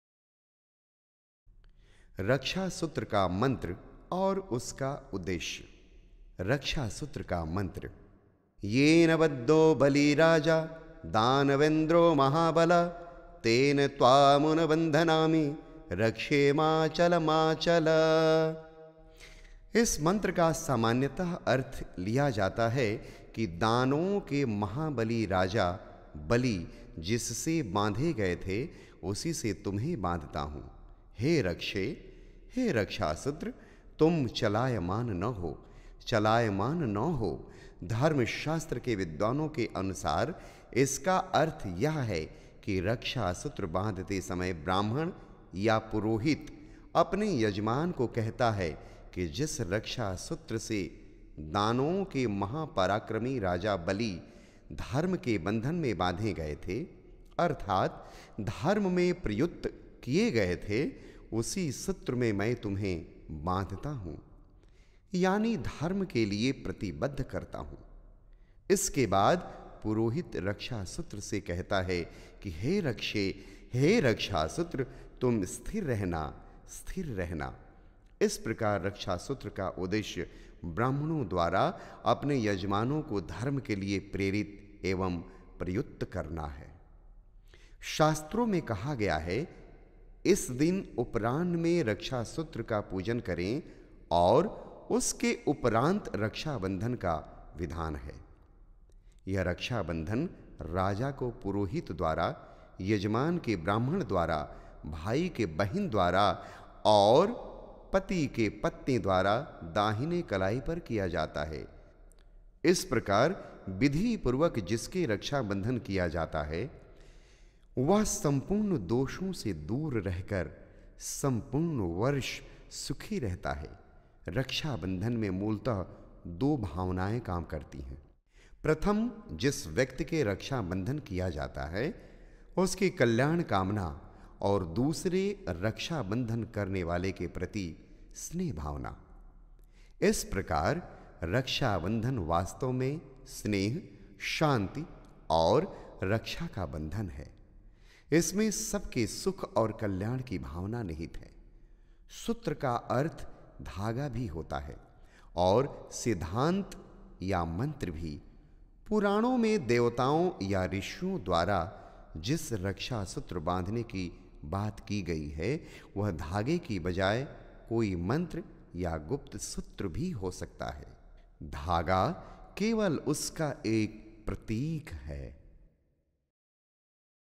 रक्षा बंधन का मंत्र और उद्देश्य | Raksha Bandhan Ka Mantra | Kamlesh Upadhyay
Title - Raksha Bandhan Ka Mantra
Narrator - Kamlesh Upadhyay